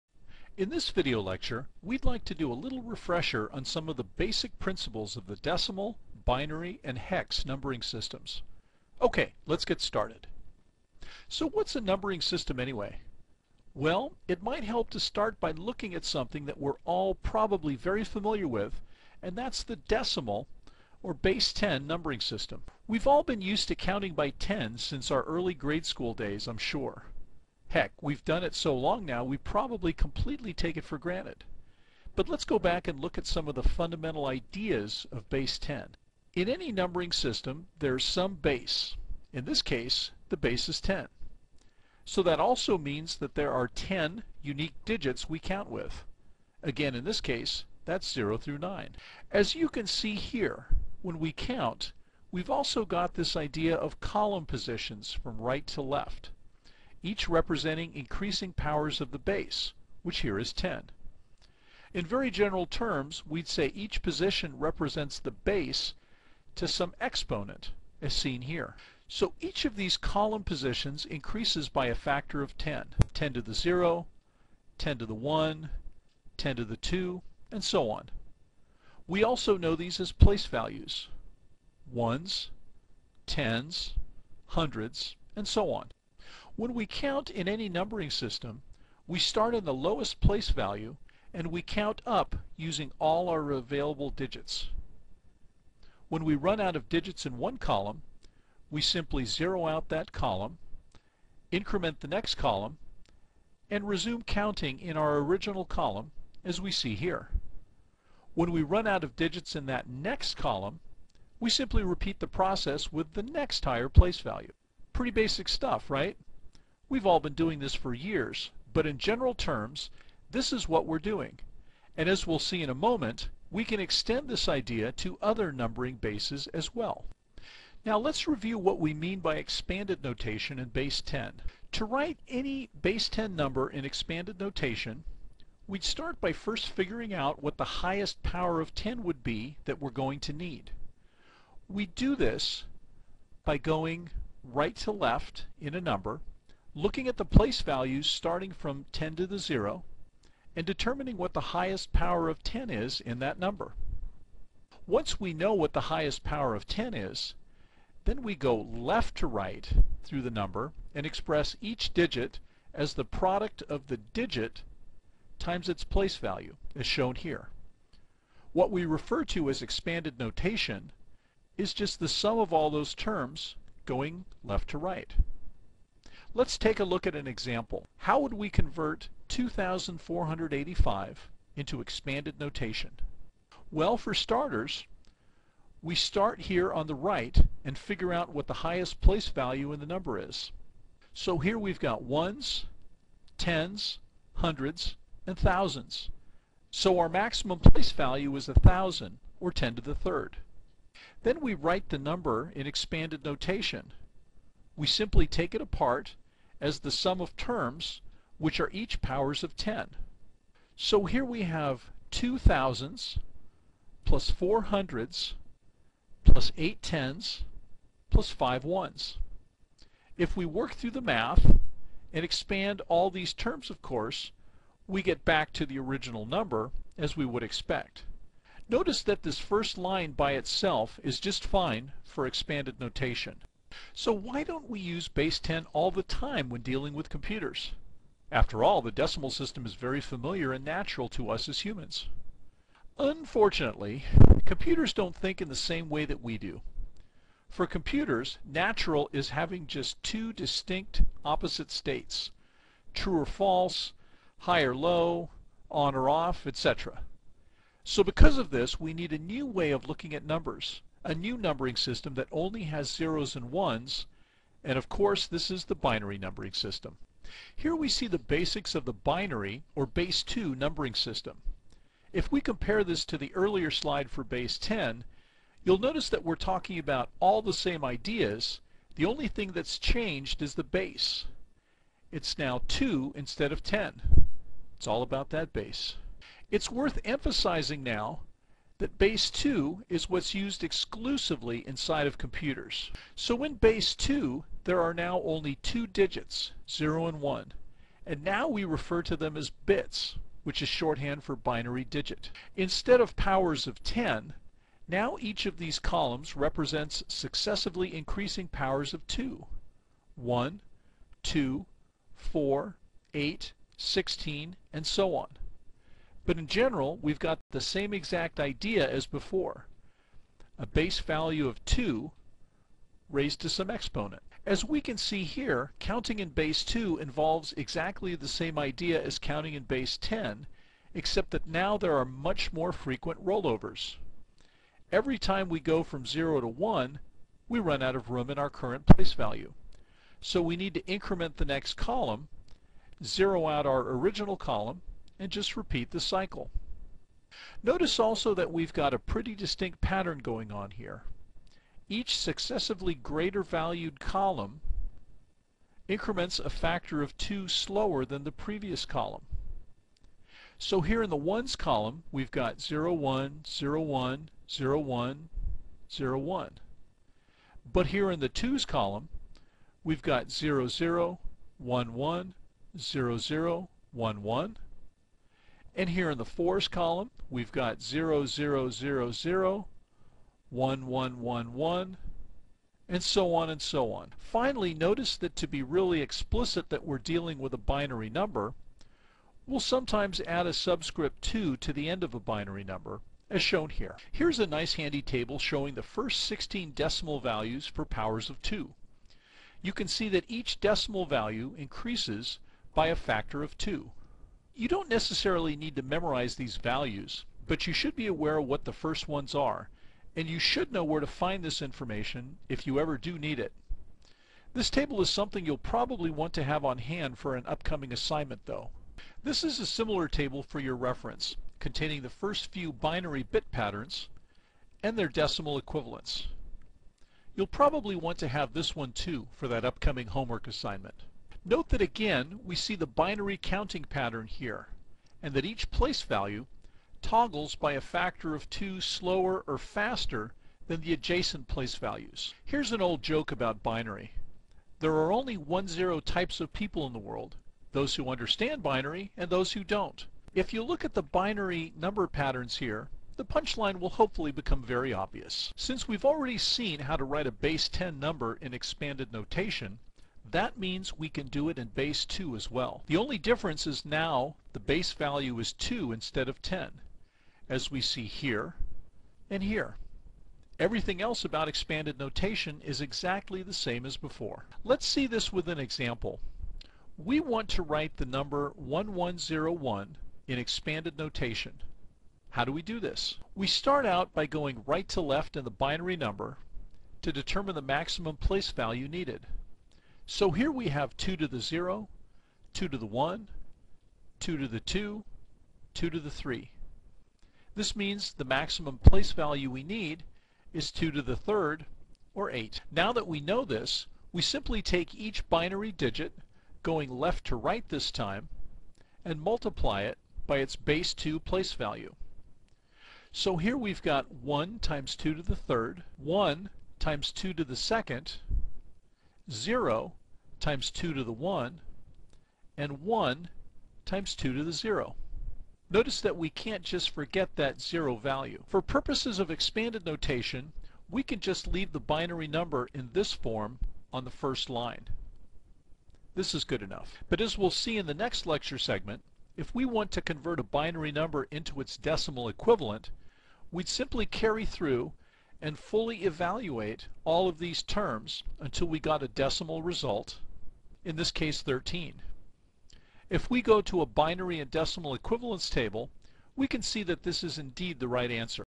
number systems 1
Basics of numbering systems, including decimal and binary, 1 of 2